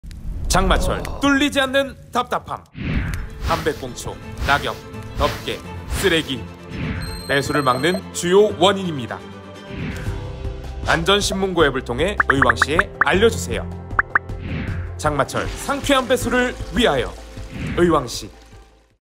누가 좀 뚫어줬으면
장마철 막힘없는 빗물받이 만들기~! 의왕시와 함께해요~!
쓰레기, 낙엽, 담배꽁초 등으로 막히거나 덮개가 덮힌 빗물받이를 보신다면
안전신문고 앱으로 의왕시에 신고해주세요~!
장마철 배수에 큰 힘이 됩니다~!